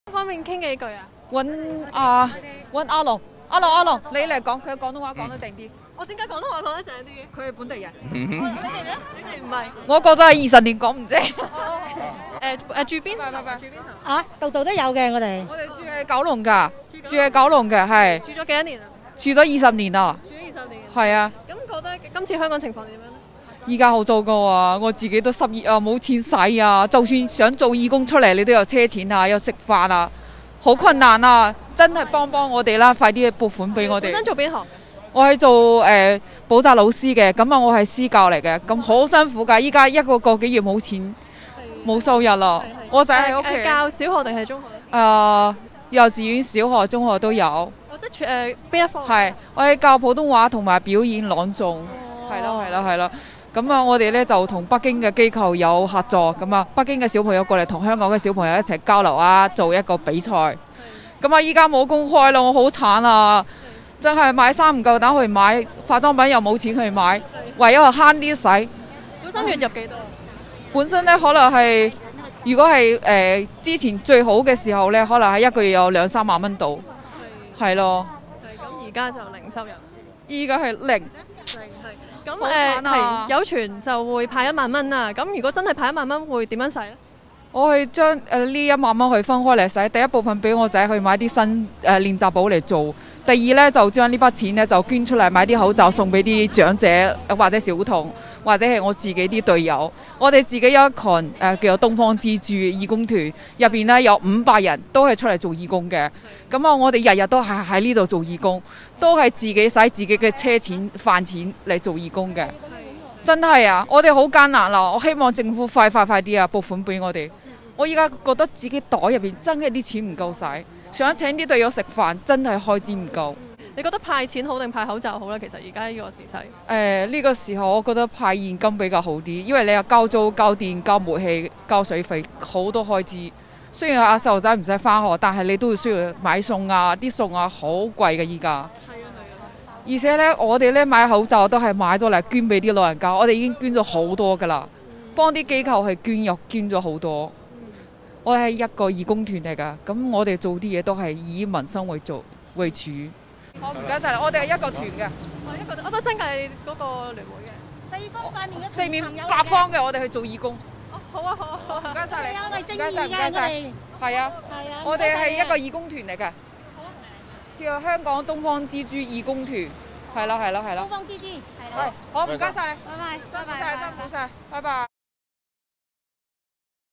民間義工團立會外請願 何老師﹕「依家好糟糕，失業無錢洗」
今早帶領「香港東方之珠義工團」出席新界社團聯會於立法會門外請願的何老師，雖然怯於自己「廣東話講得唔正」，但仍希望表達「派一萬元」的訴求。聲稱來港二十年、從事私人補習、教授普通話的何老師坦言，「依家好糟糕，失業無錢洗」，如果獲派一萬元，「首先買練習簿俾個仔，之後買口罩送俾人」。